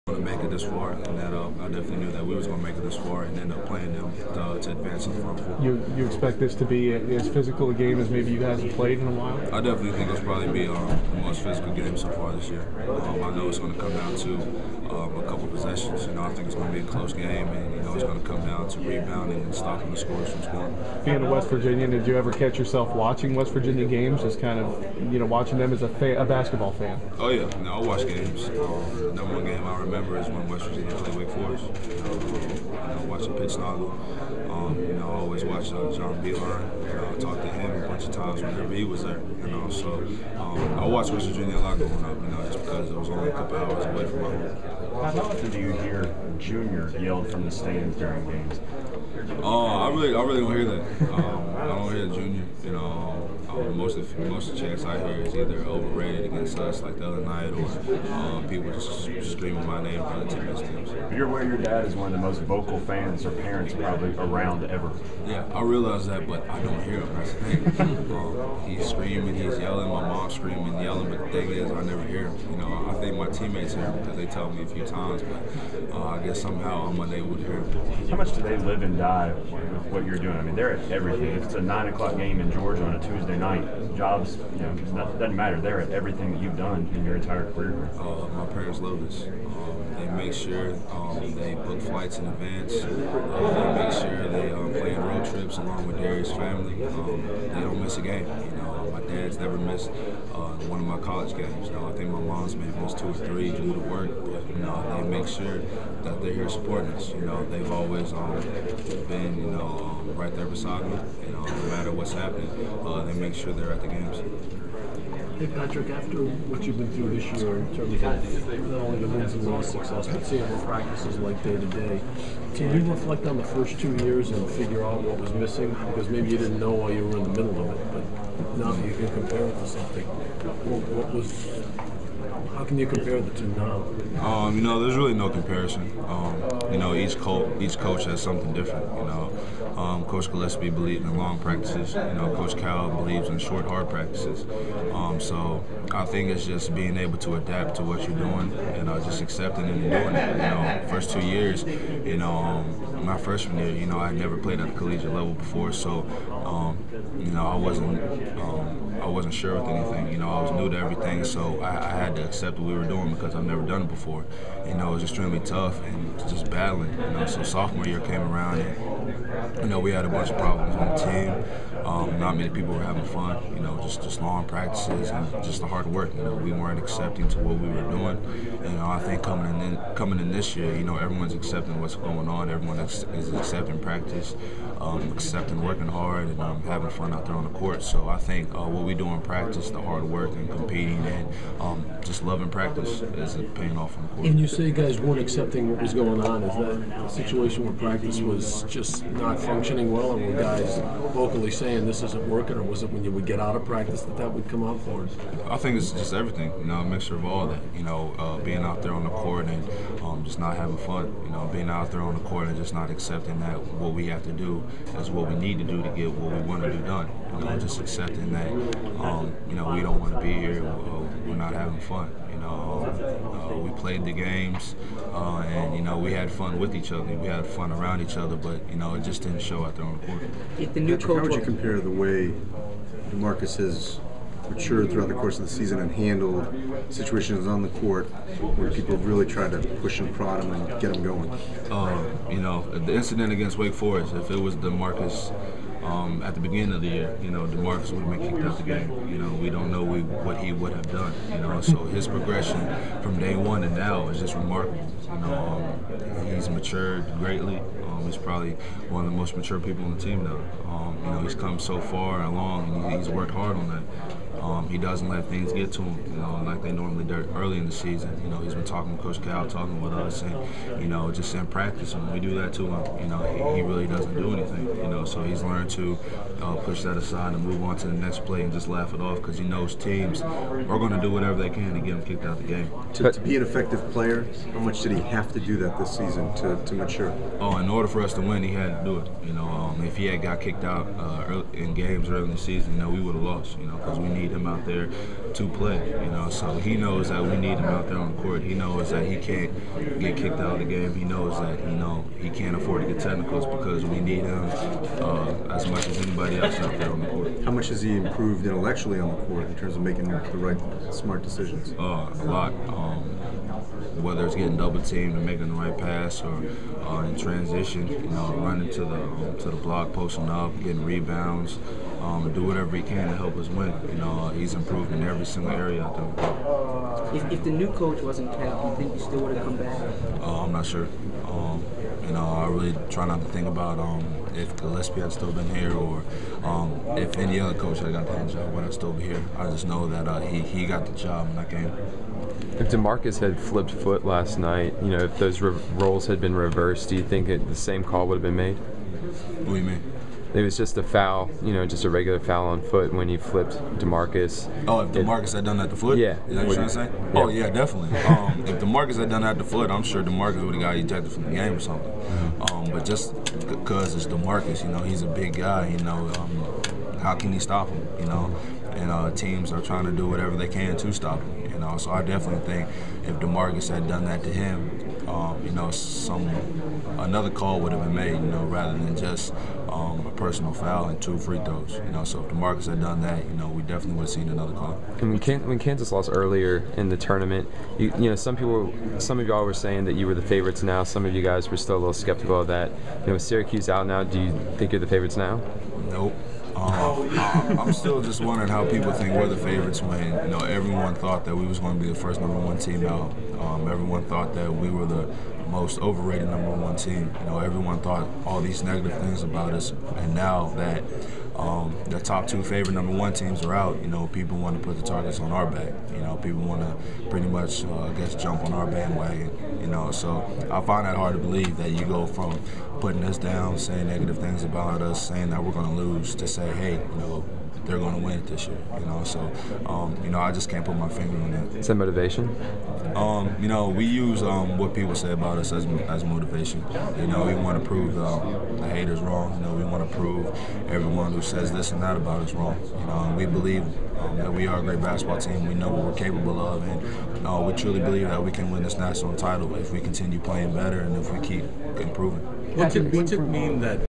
Patterson talks about WV, last year
Junior forward Patrick Patterson talks about going against West Virginia and compares last season to this season.